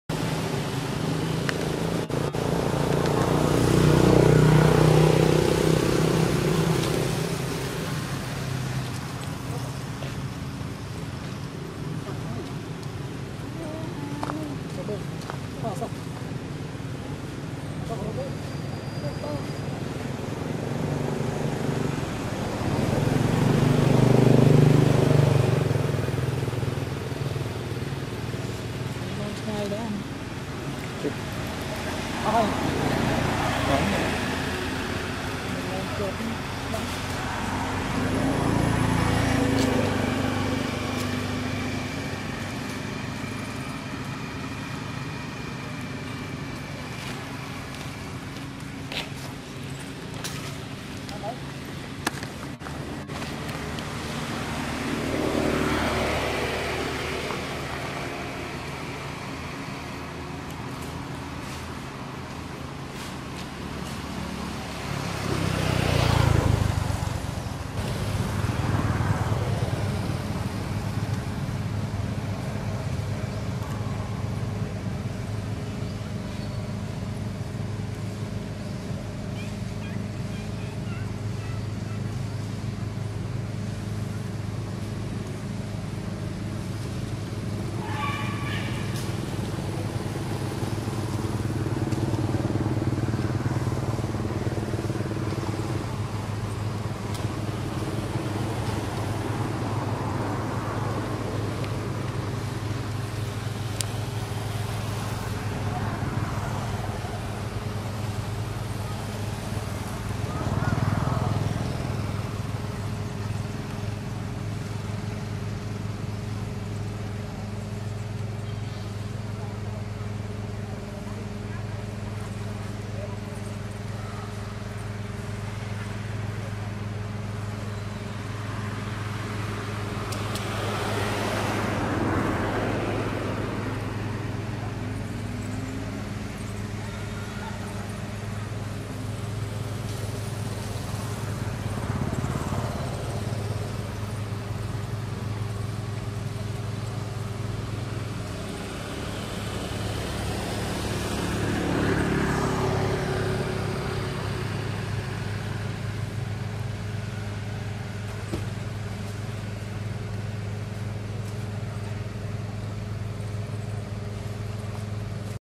Monkey Jessie Lost for months Agos but Dee Dee - Maria And Sankra live together
Monkey Jessie Lost for months Agos , Dee Dee - Maria And Sankra live together